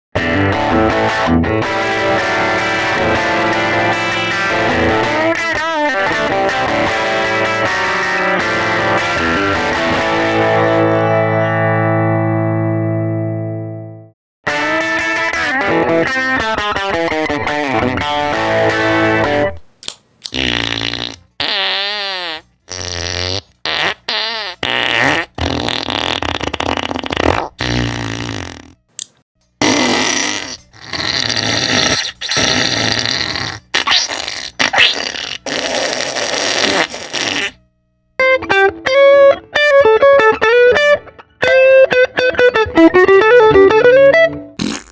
Guitarist Dag Juhlin demos The Fart Pedal
Guitarist Dag Juhlin (The Sunshine Boys, Poi Dog Pondering) demonstrates The Fart Pedal in both dry and wet mode.